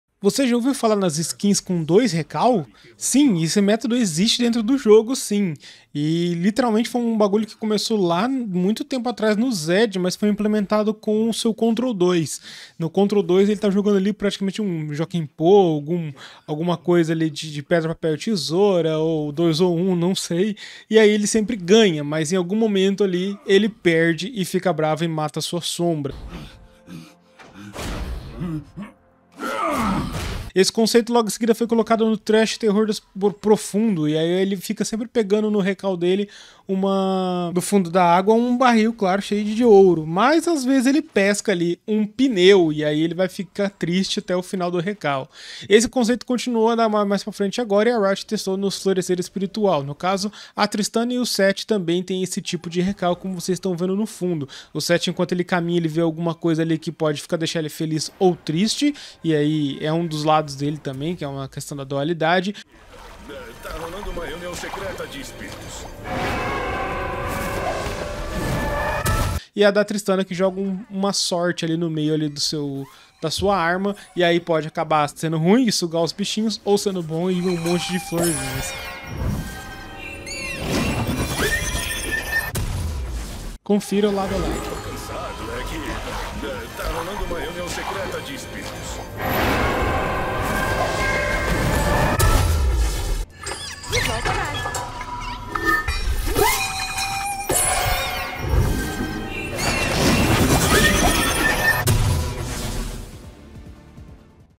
SKINS COM 2 RECALLS DIFERENTES
Confira os campeões que tem 2 animações diferentes pra recall ou Controls